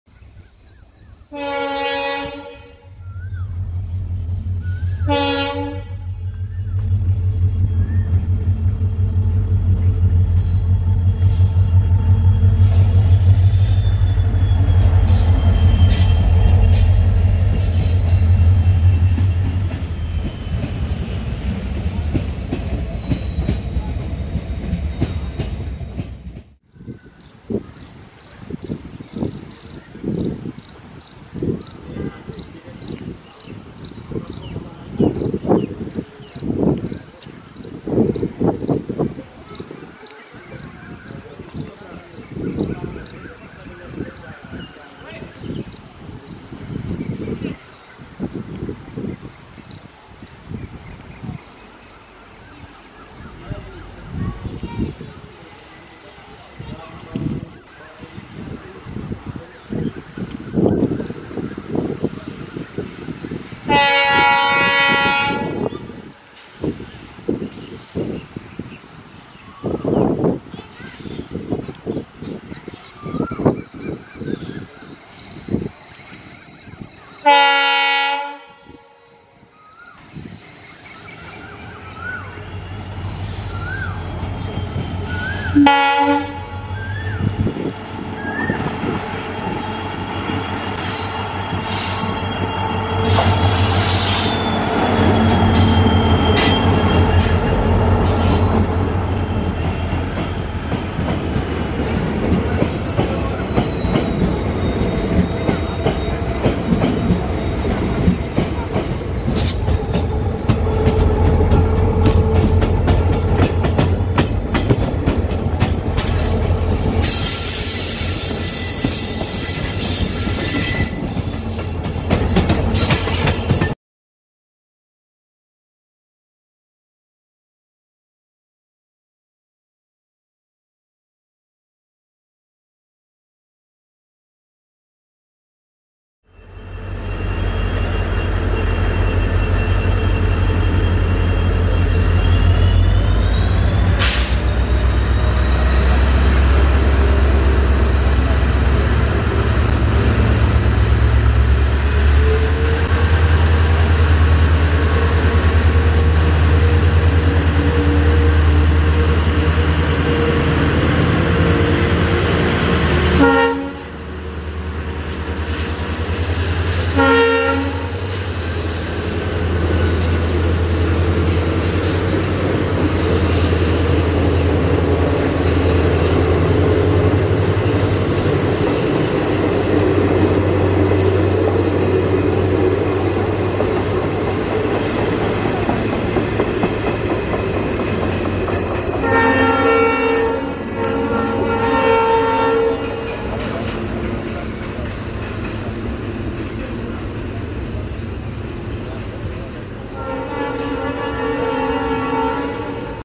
Nilgiri Locomotive || Freshly Painted Tkd Emd Locomotive
NILGIRI Moving Mountains || A Dedicated Locomotive To Mountains By Diesel Loco Shed Tughlakabad

12248 TKD WDG4 With 04352 Hisar-Delhi Special